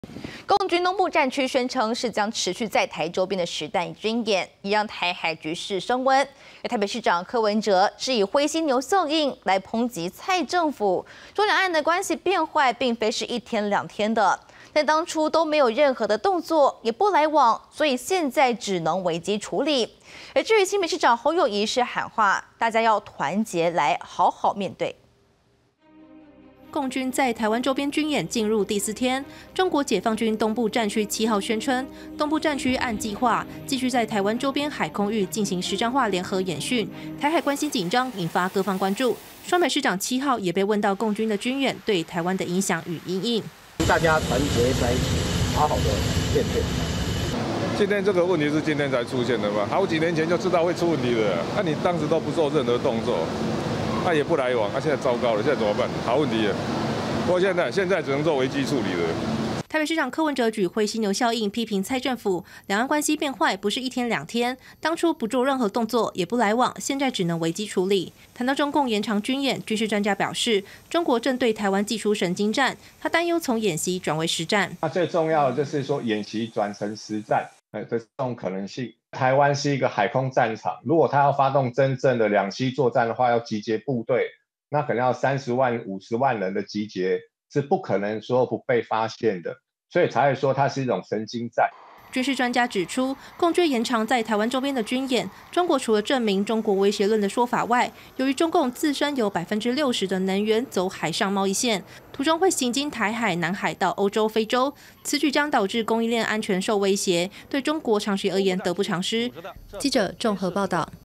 中宣稱將持續對台軍演 柯文哲舉「灰犀牛效應」批蔡政府｜20220807公視晚間新聞
共軍東部戰區宣稱將持續在台灣周邊的實彈軍演，升高台海緊張，引發關注，台北市長柯文哲舉灰犀牛效應批評蔡政府，兩岸關係變壞不是一天兩天，當初都不做任何動作，也不來往，現在只能危機處理，新北市長侯友宜則喊話大家團結，好好面對。